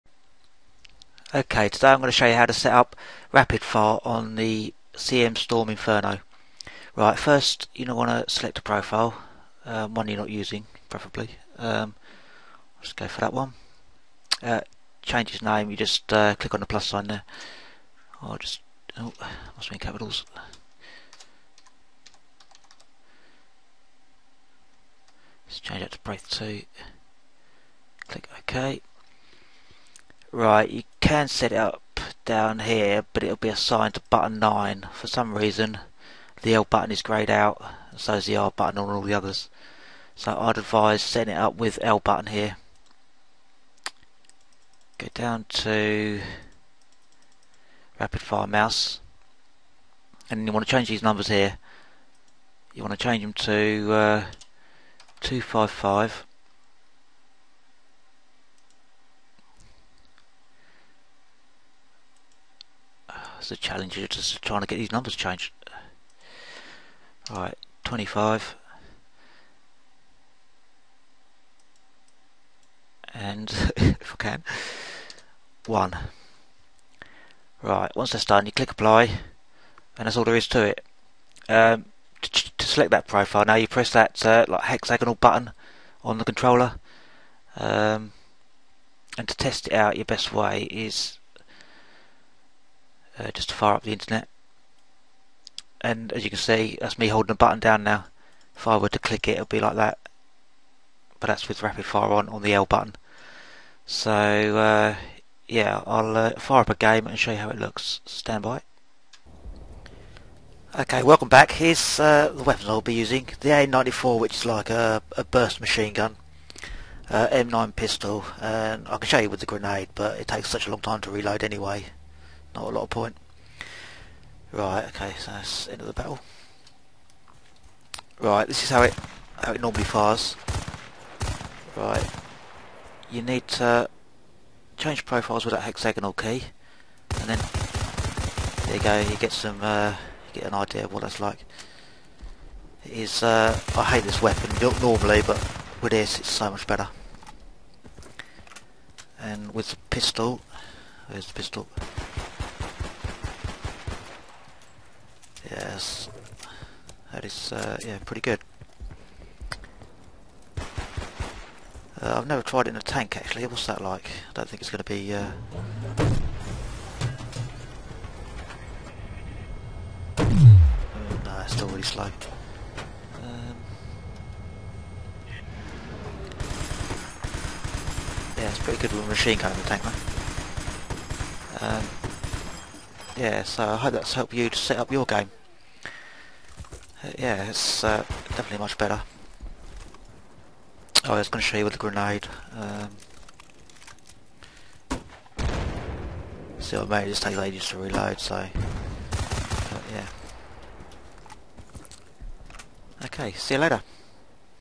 Setting up Rapid Fire on CM Storm Inferno
Its all in the video but the settings I used are 255 25 1